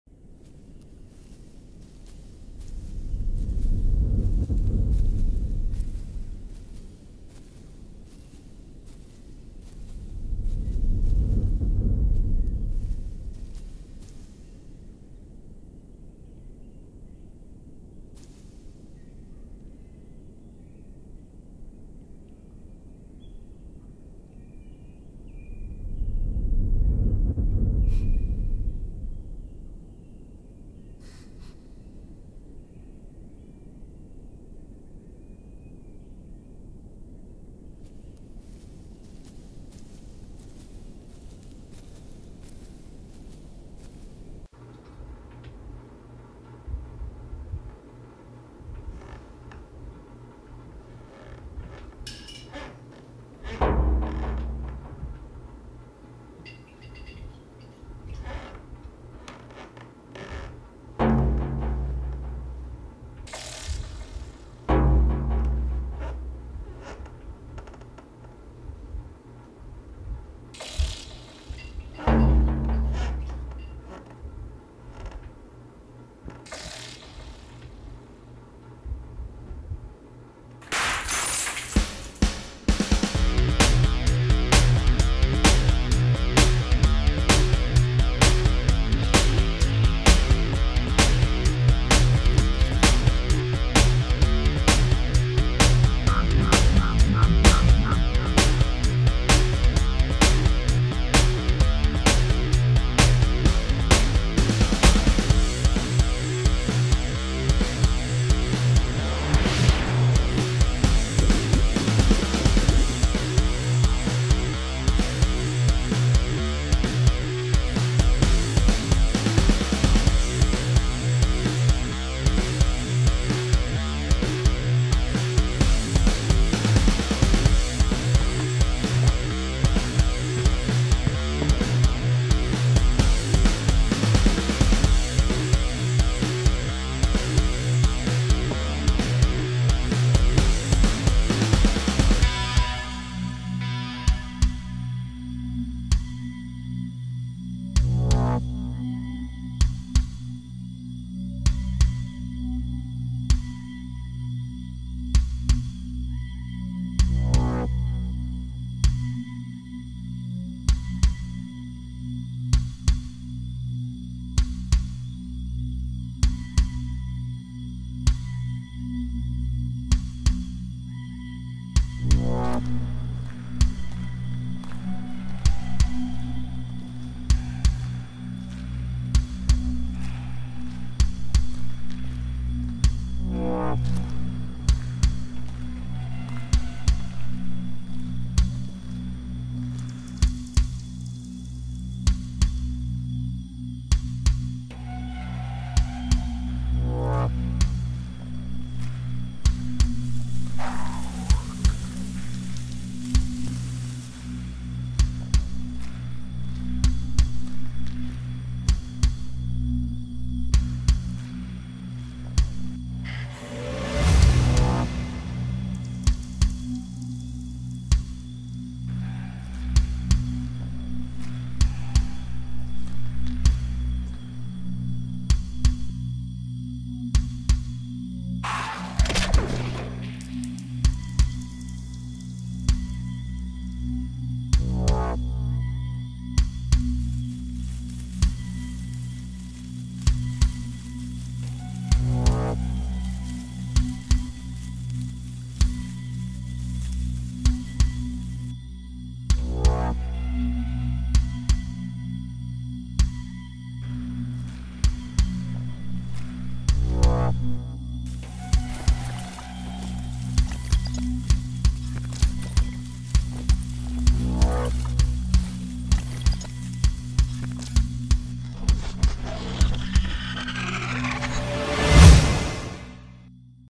Face à mon destin ( court métrage )
Ayant perdu ma femme et mon enfant suite à une explosion nucléaire, je décide de prendre mon courage à deux mains et faire face à mon destin.